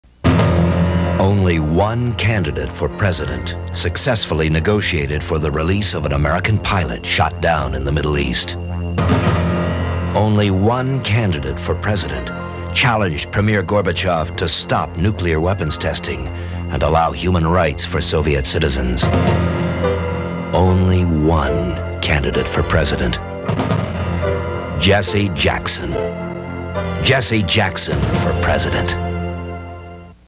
Jesse Jackson [Democratic] 1988 Campaign Ad "Only One Candidate"
Jesse Jackson, Democratic candidate for United State President. This 1988 campaign ad entitled "Only One Candidate" depicts Jesse Jackson negotiating release of hostage, stopped nuclear weapons testing.

Creator: Other

Location: Other

Format: 3/4" film; video/mp4

Extent: 0 minutes 31 seconds

Subjects: Patriotism; Weapons